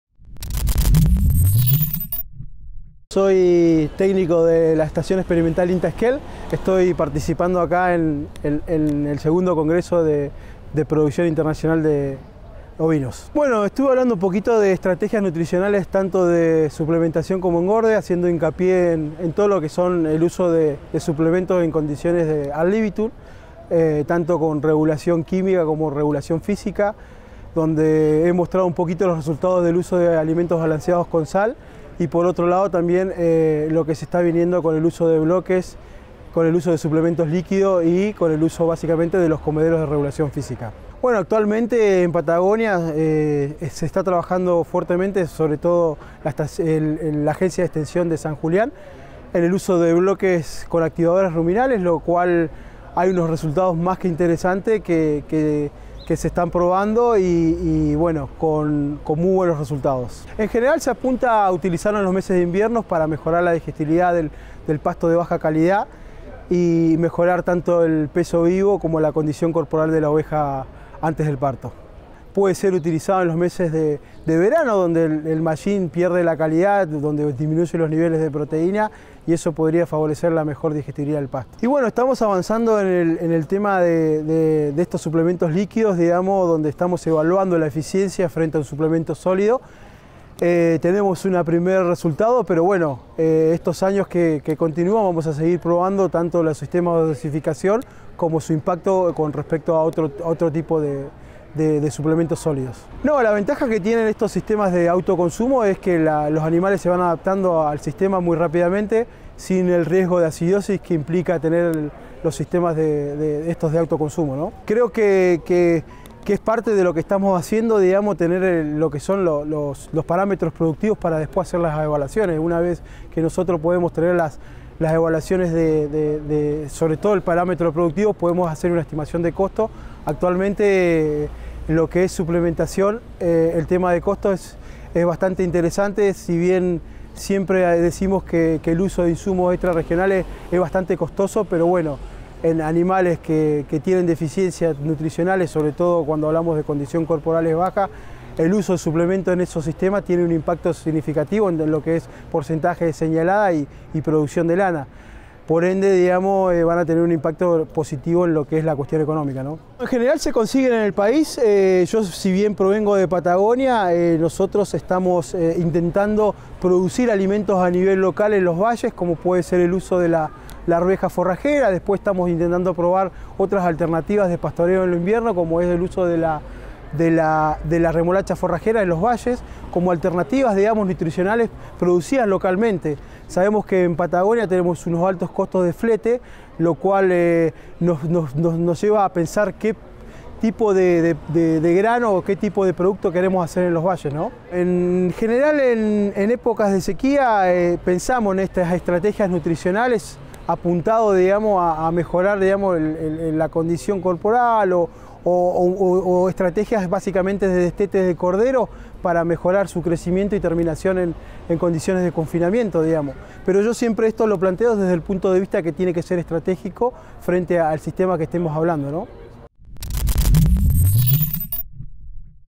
Estrategias de nutrición ovina
El Ing. Demian Ceballos de INTA ESQUEL, cuenta detalles de suplementación y de engorde con distintas variantes.